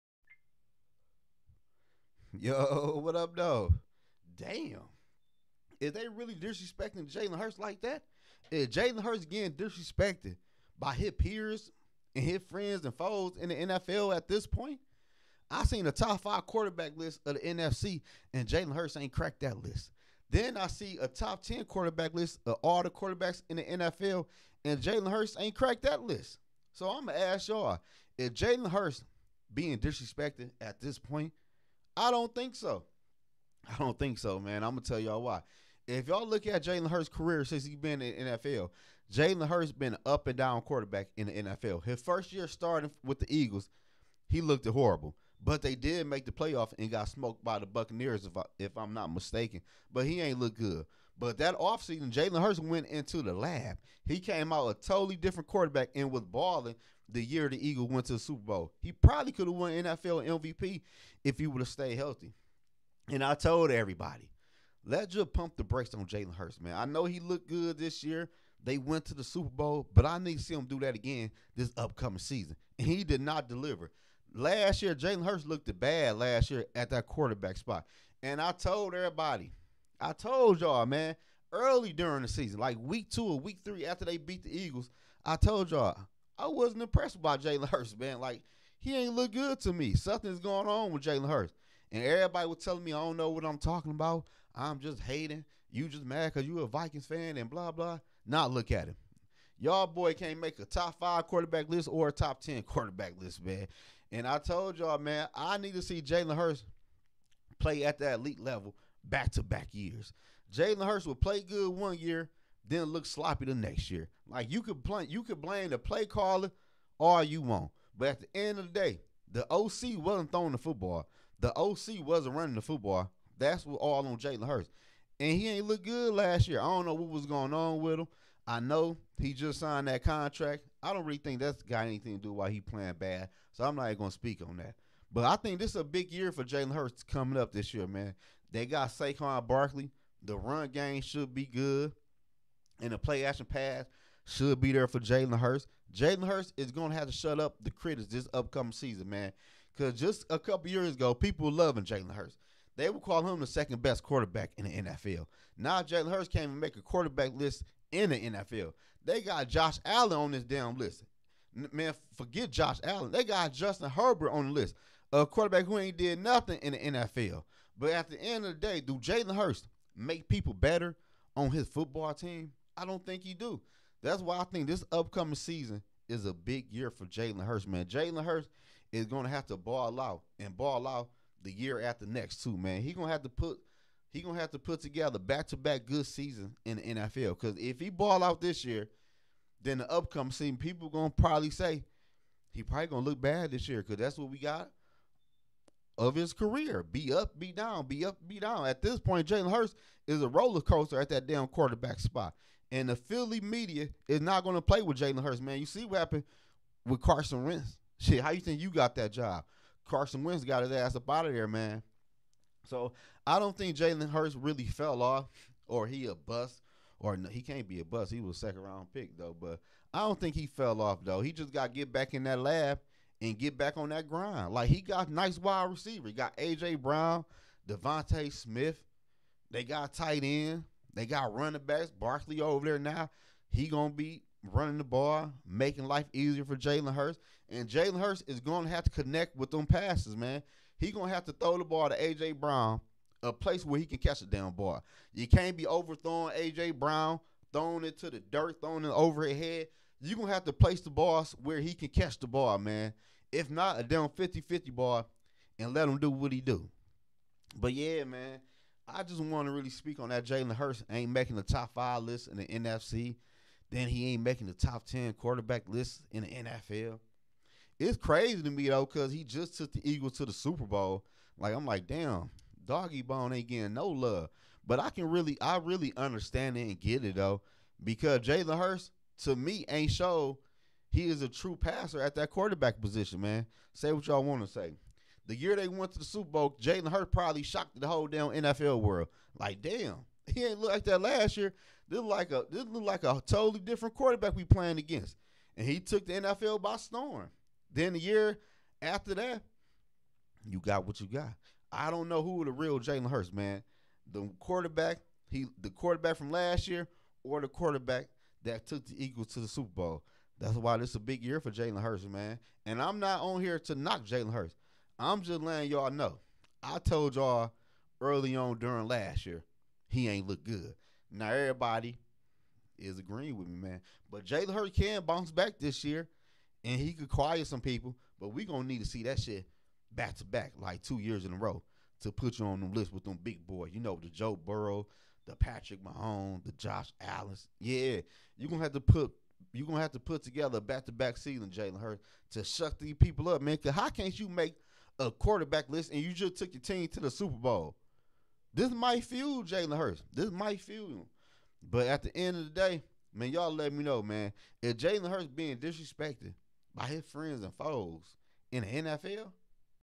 Jalen Hurts being DISRESPECTED by the NFL
Jalen Hurts didn't make the top 10 quarterbacks list in the NFL.